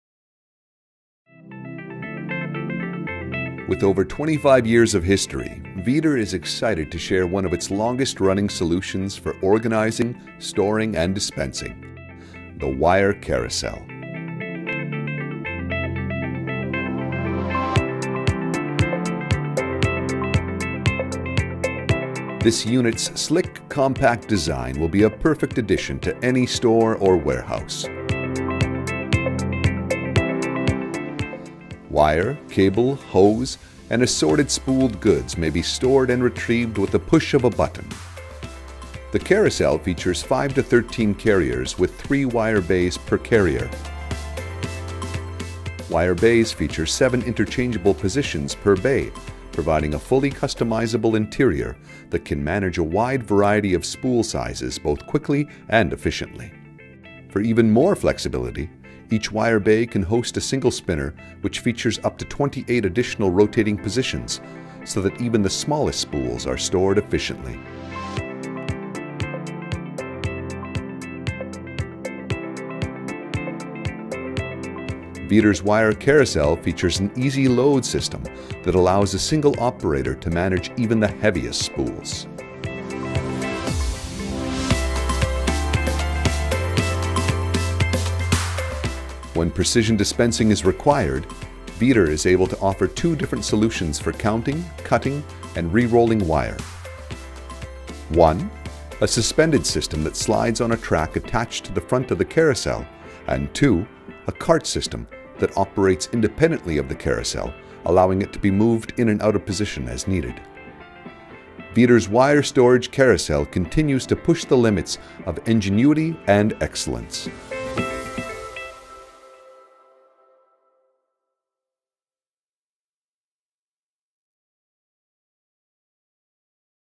Wire Carousel Video
The wire and spool carousel allows you to consolidate spool storage in a small footprint by utilizing vertical height. The configuration is flexible to meet your needs, storing up to 36 x 30" spools. Spinner inserts allow you to store up to 24 small spools on a single carrier.